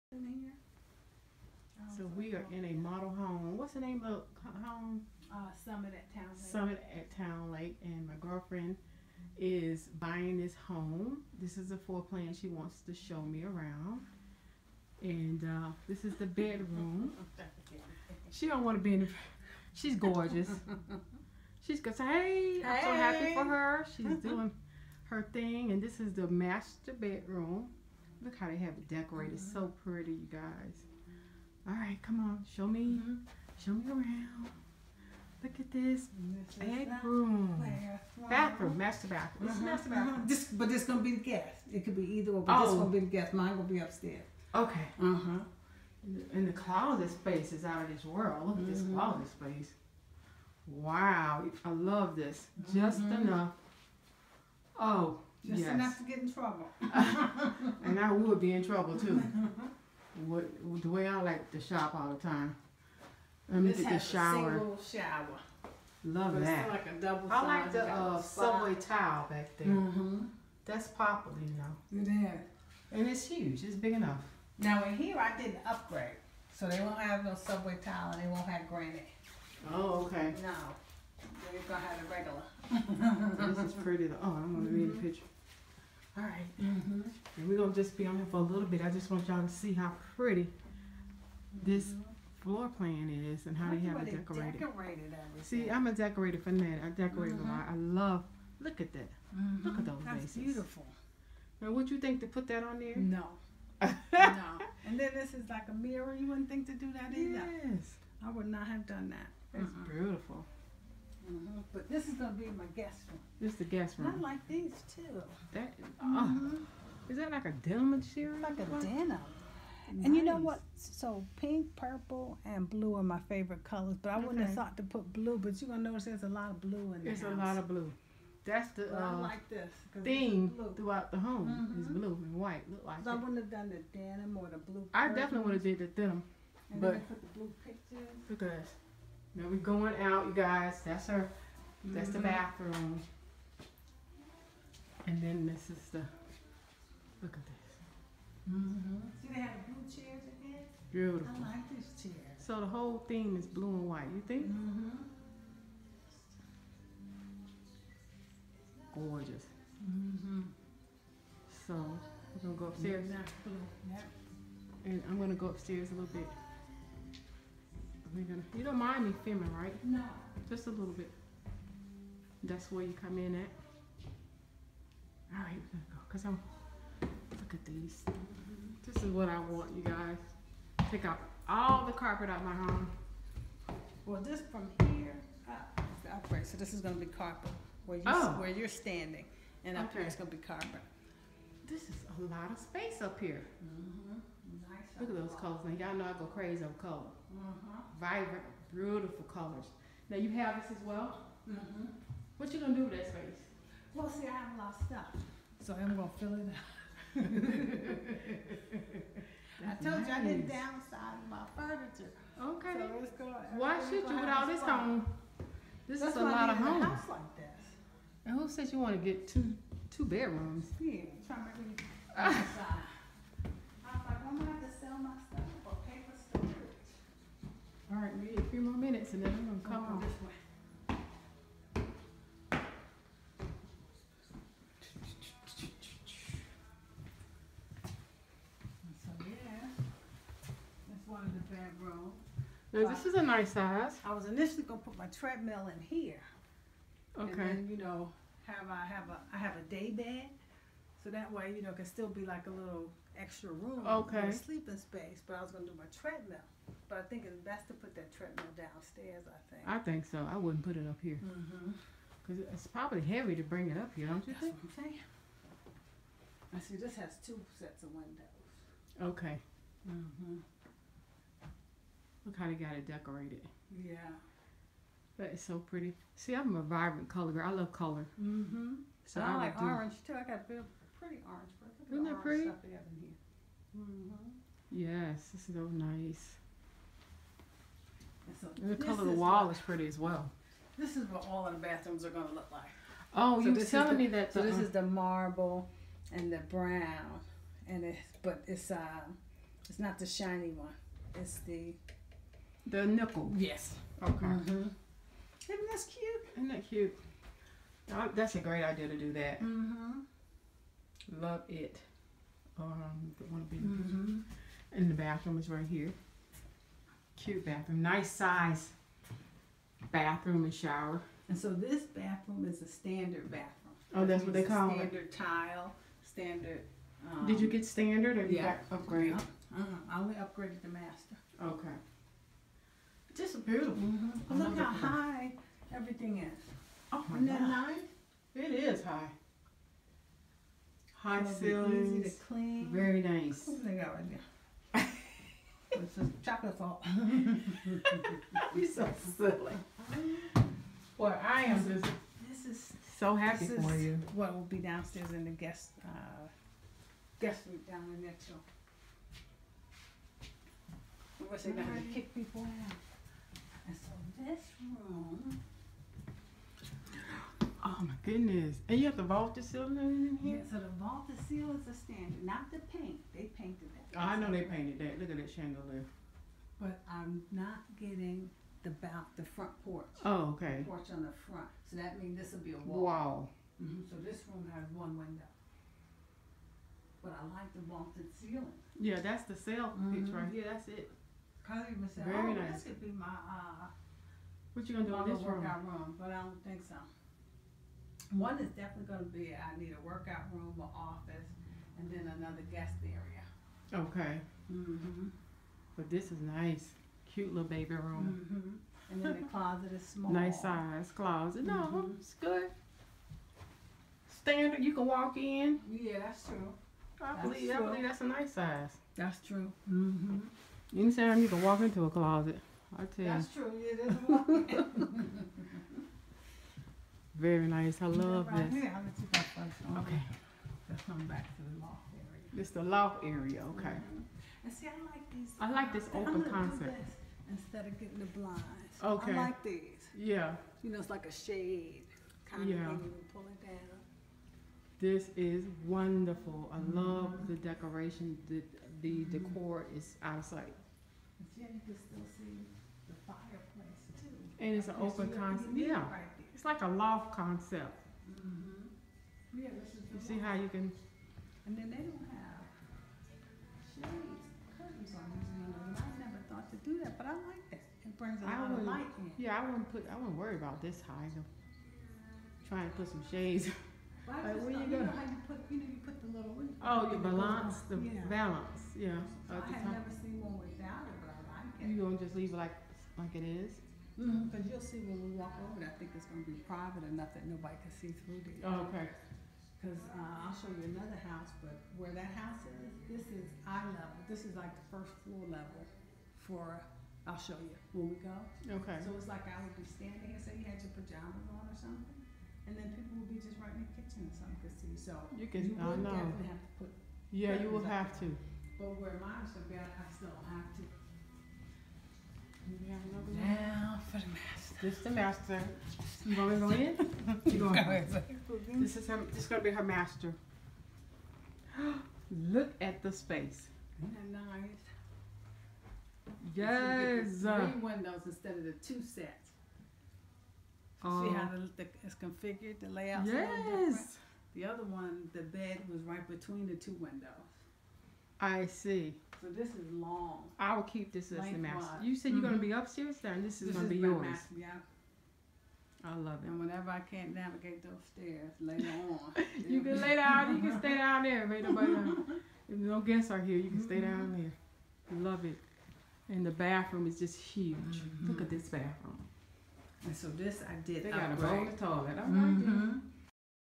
GOALS MODEL HOME 🏡 TOUR
I am so happy for my girlfriend she has purchase a home in my area so we are taking a walk through look at her floor plan with in the model home this is my goal as well to move it to another home so I’m happy that she allowed me to share with you please leave a comment subscribe and we have not and thumbs up the video thank you for watching